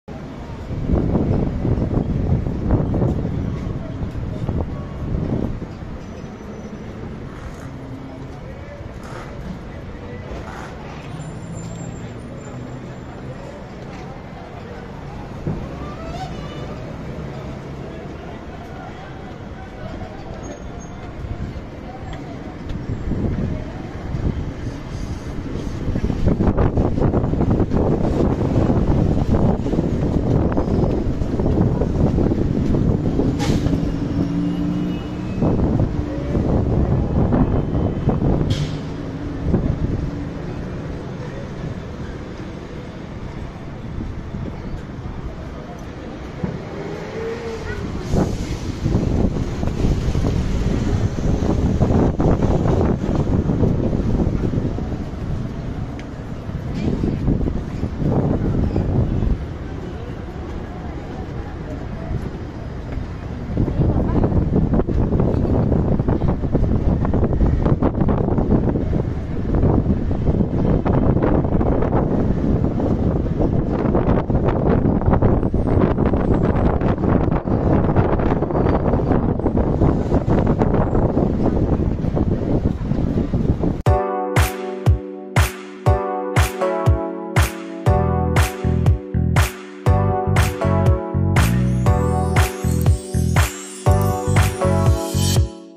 Concarril FM 86 (FM.18/FM.17) Tren En Contrasentido, Haciendo Maniobra SP (4K UHD)
22 Mayo 2022

Comenta, Si Te Gusta Ver, Los Trenes Haciendo Maniobras.